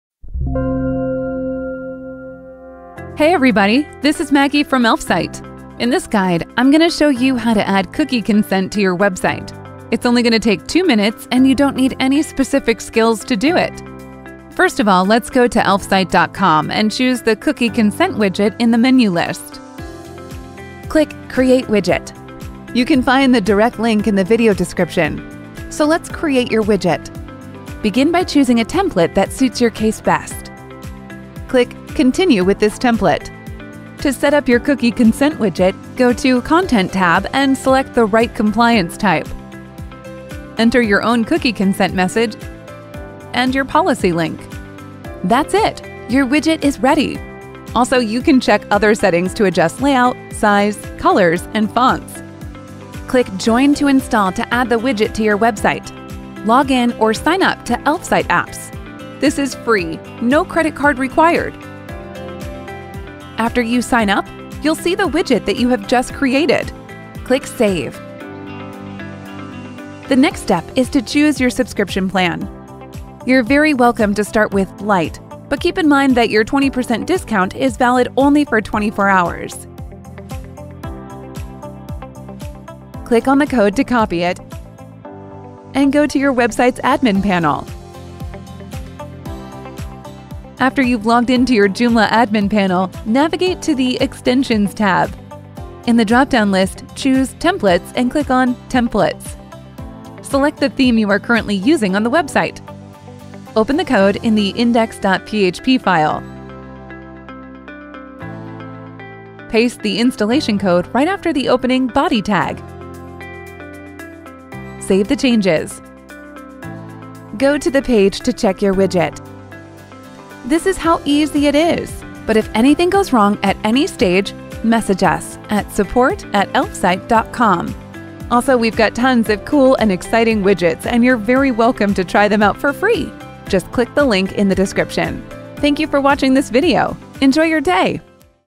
How to Add Cookie Consent Extension to Joomla
Add Cookie extension to Joomla site in just 2 minutes -

Cookie Consent is a free tool to make an instant cookie notification with a consent button. Inform your website visitors about utilizing cookies, set up and control the widget without any coding skills.

Attributes:

• Customizable color of buttons and links.
• 2 types of approval demands: Decline and Accept buttons.
• left, center, and right aligns and 2 layouts: Box and Bar.

How to embed Cookie Consent on Joomla website?

In order to add our extension, follow all the actions from the brief guideline below:

1. Generate and configure the extension;
2. Save a unique code which you will see in the notification popup;
3. Add the copied code on the site.

Check how to add Joomla Cookie Banner plugin here: -

Best Cookie extension integration templates:

Example 1: Top Banner.
Example 2: Center Floating Box.
Example 3: Bottom Banner.

This is how smooth it is to integrate the Cookie Banner plugin to Joomla! If you wish to learn more about extension templates, settings, and benefits, you can contact our dedicated Support team. They’ll eagerly answer all your queries!